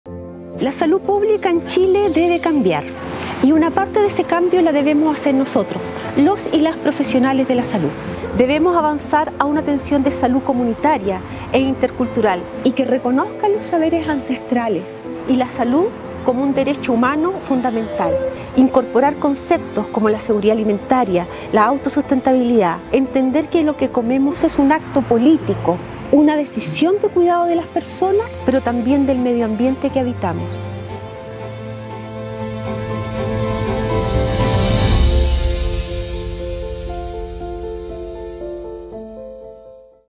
Ana María Neira, jefa de Nutrición y Dietética de la Academia.
Ana María Neira, jefa de la carrera de Nutrición y Dietética de la Academia, nos habla del enfoque de las nuevas carreras de Salud de la Universidad.